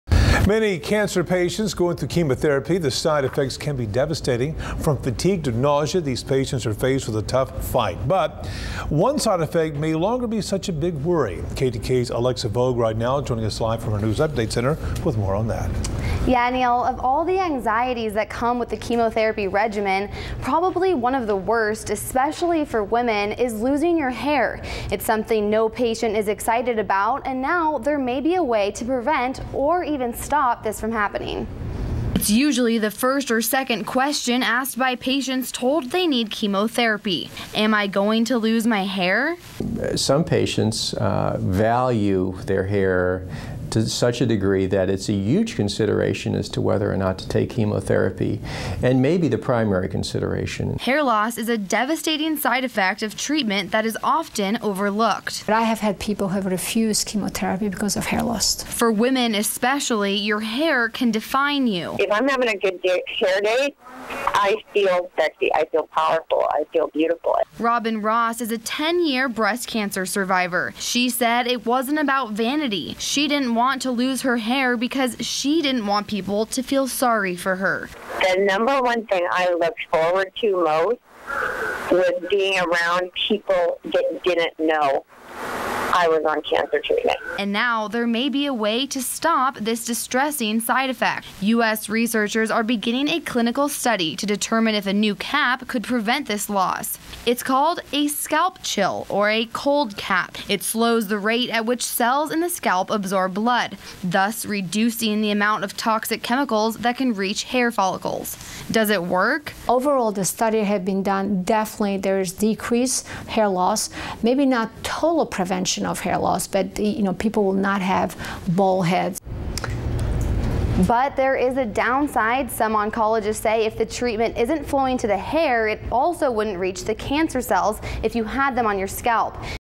Can hair loss in chemotherapy patients be prevented?
TYLER, TX (KETK) - It's usually the first or second question asked by patients told they need chemotherapy.

'Am I going to lose my hair?'

"Some patients value their hair to such a degree that it's a huge consideration as to whether or not to take chemotherapy," said Dr. Edward Sauter. "And maybe the primary reason."

Hair loss is a devastating side effect of treatment that is often overlooked.

"I have had people who have refused chemotherapy because of hair loss," said Dr. Sasha Vukelja at Texas Oncology-Tyler.

For women especially, your hair can define you.

"If I'm having a good hair day I feel sexy I feel strong I feel beautiful," said Robin Ross, ten year breast cancer survivor.

She said it wasn't about vanity.

She didn't want to lose her hair because she didn't want people to feel sorry for her.

"The thing I looked forward to most was being around people who didn't know I was on cancer treatment," Ross said.

And now there may be a way to stop this distressing side effect.

U.S.. Researchers are beginning a clinical study to determine if a new cap could prevent this loss.

It has been called a 'scalp chill' or a 'cold cap.'

It slows the rate at which cells in the scalp absorb blood, thus reducing the amount of toxic chemicals that can reach hair follicles.

"Overall, the studies that have been done definitely there is a decrease in hair loss," Dr. Vukelja said. "Maybe not total prevention of hair loss, but people will not have bald heads."

But, there is a downside.

Some oncologists say if the treatment is not flowing to the hair, it also would not reach cancer cells on your head.